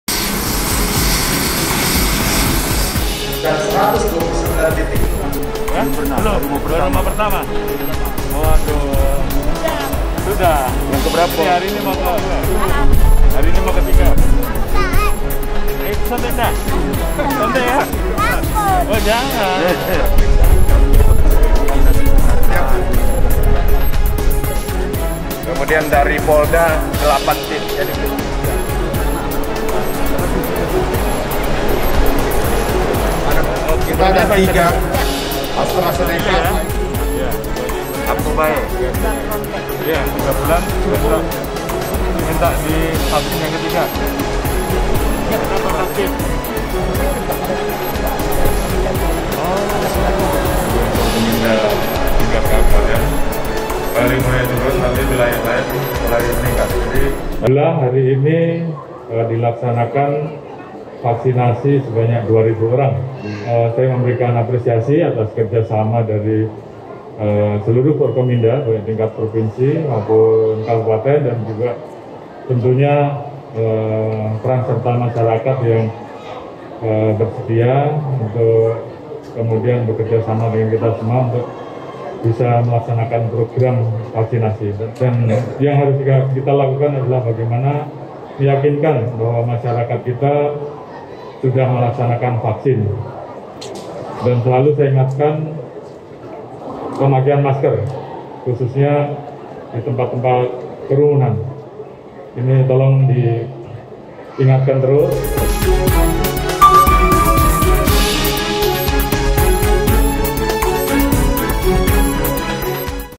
Tinjau Vaksinasi, Kapolri Dorong Percepatan Target Dosis Dua dan Booster
Kapolri meninjau pelaksanaan akselerasi vaksinasi di GOR, Kajang, Kayu Agung, Ogan Komering Ilir (OKI), Sumsel, Kamis (24/2/2022). Kegiatan vaksinasi juga dilakukan serentak di 34 Provinsi. kapolri juga memberikan pengarahan secara langsung kepada seluruh wilayah di Indonesia.

Android: inews.id/android
iPhone: inews.id/ios